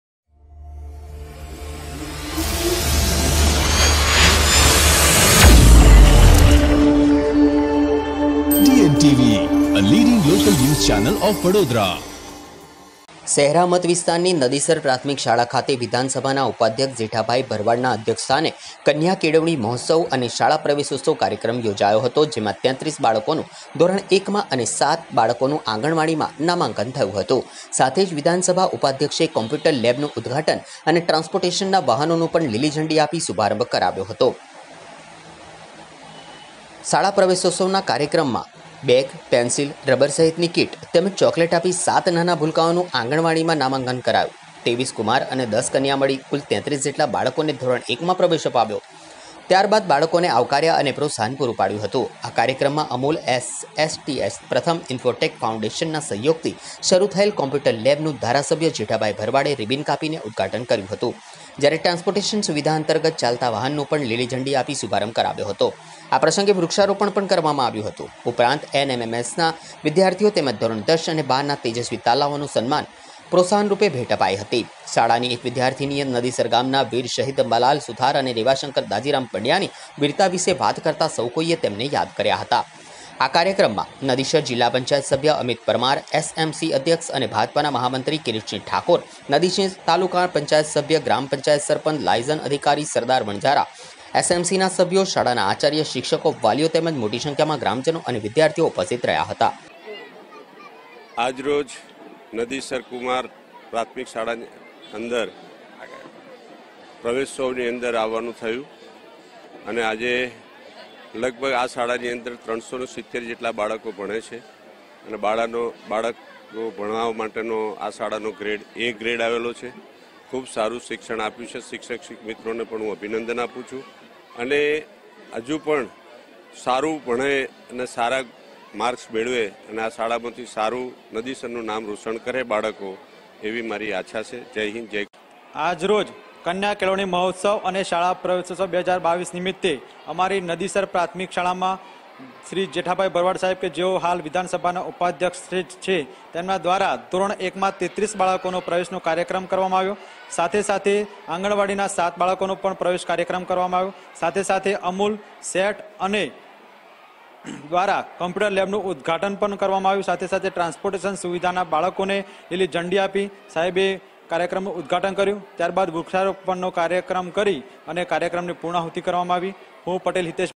26/06/22 શહેરા મતવિસ્તારની નદીસર પ્રાથમિક શાળા ખાતે શાળા પ્રવેશોત્સવ કાર્યક્રમ યોજાયો
26/06/22 શહેરા મતવિસ્તારની નદીસર પ્રાથમિક શાળા ખાતે વિધાનસભા ઉપાધ્યક્ષ જેઠાભાઈ ભરવાડના અધ્યક્ષસ્થાને કન્યા કેળવણી મહોત્સવ અને શાળા પ્રવેશોત્સવ કાર્યક્રમ યોજાયો